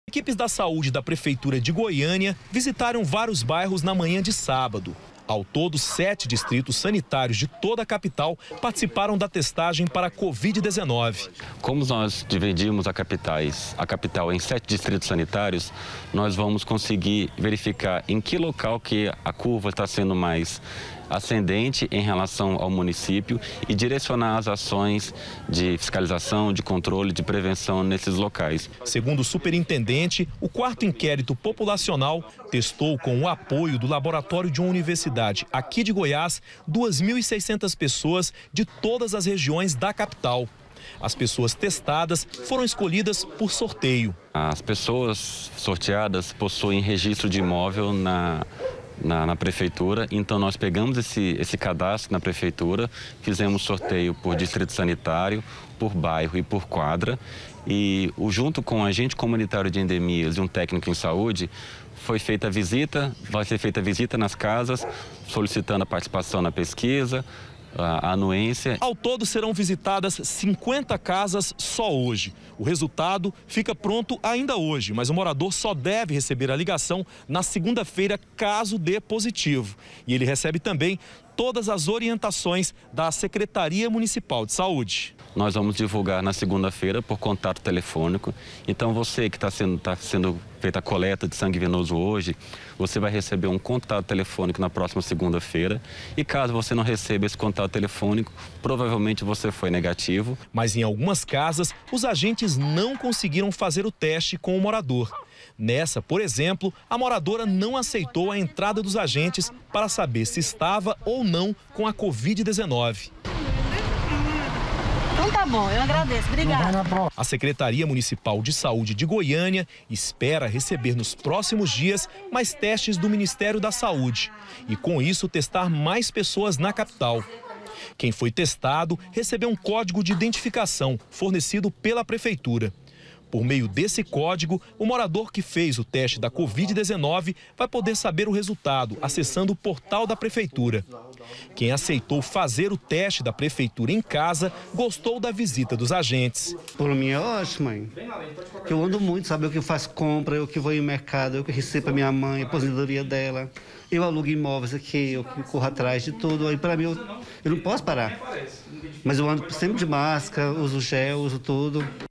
COVID-19: PREFEITURA FAZ TESTE COM 2.600 PESSOAS
Cerca de duas mil e seiscentas pessoas estão fazendo testagem para covid-19. O serviço está sendo feito por agentes de saúde da prefeitura de Goiânia. O resultado deve sair até na próxima segunda-feira.